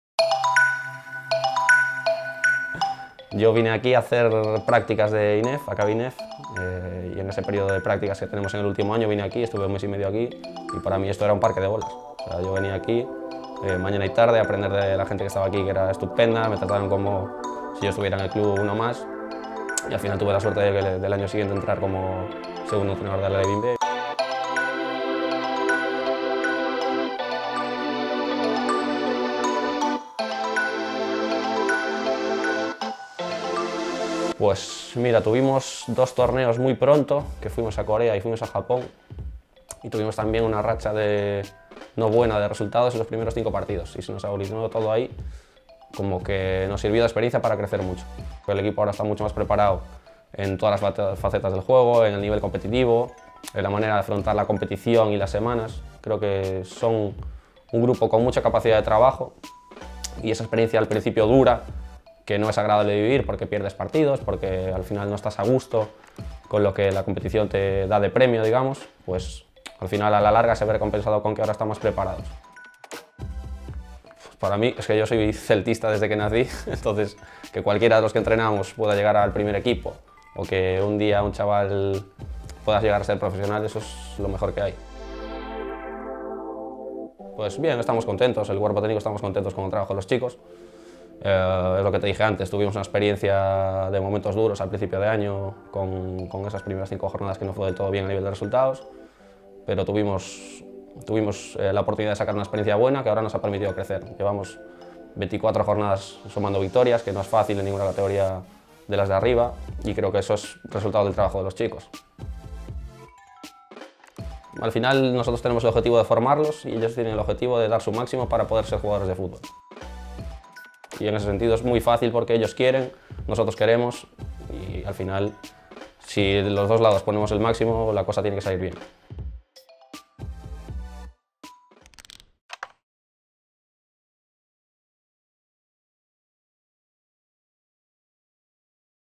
Coñecendo A Canteira - Adrián Abalo (Cadete A)
Novo episodio da serie 'Coñecendo A Canteira', na que coñecemos a canteira celeste da man dos seus protagonistas. Nesta ocasión, Celta Media entrevista ao adestrador do Cadete A, Adrián Abalo.